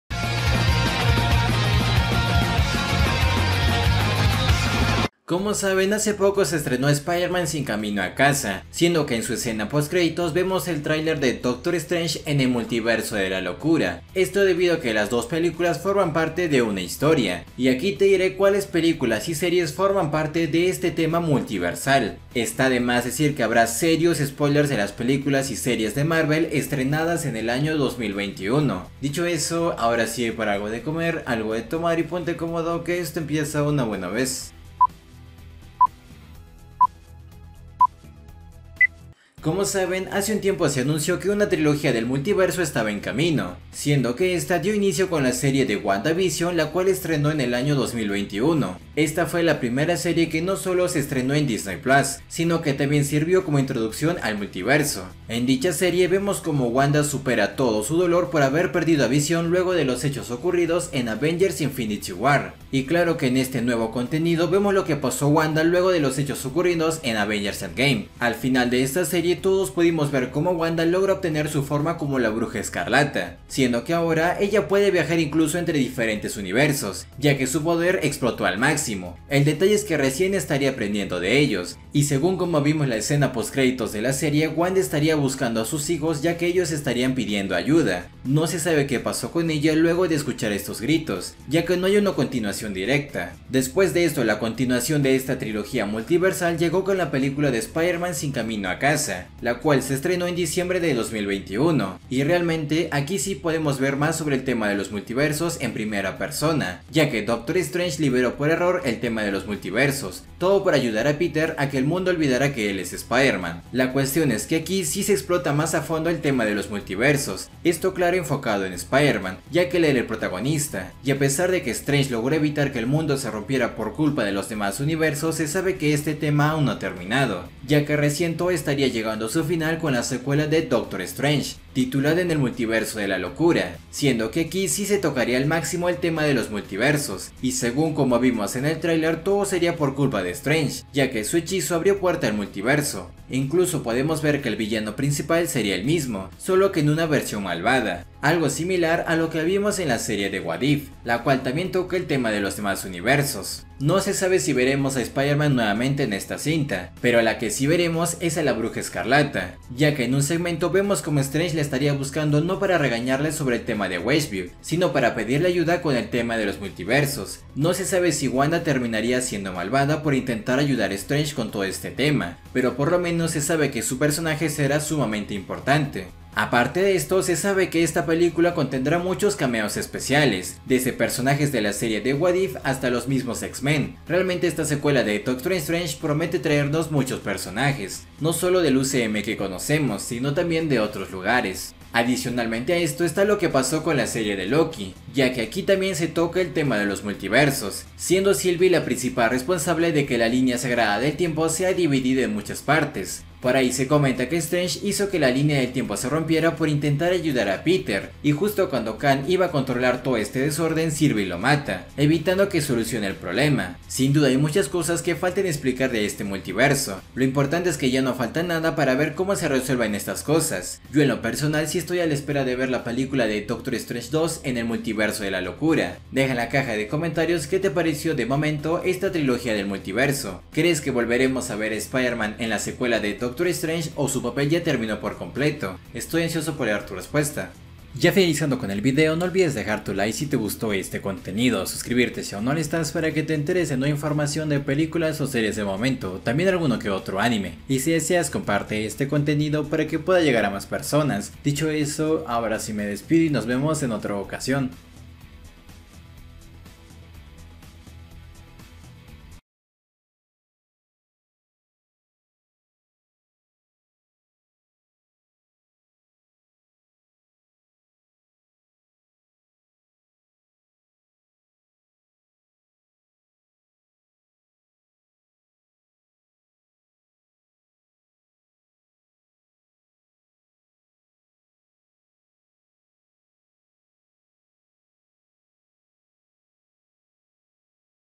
¡MULTIVERSO UCM! LA TRILOGÍA DEL MULTIVERSO CULMINARÍA EN DOCTOR STRANGER 2 - SPIDERMAN NO WAY HOME
Como saben hace poco se estrenó spiderman sin camino a casa, siendo que en su escena post créditos vemos el trailer de doctor stranger en el multiverso de la locura, esto debido a que las dos películas forman parte de una historia, y aquí te diré cuales peliculas y series forman parte de este tema multiversal, está demás decir uqe habrá serios spoilers de películas y series de marvel estrenadas en el 2021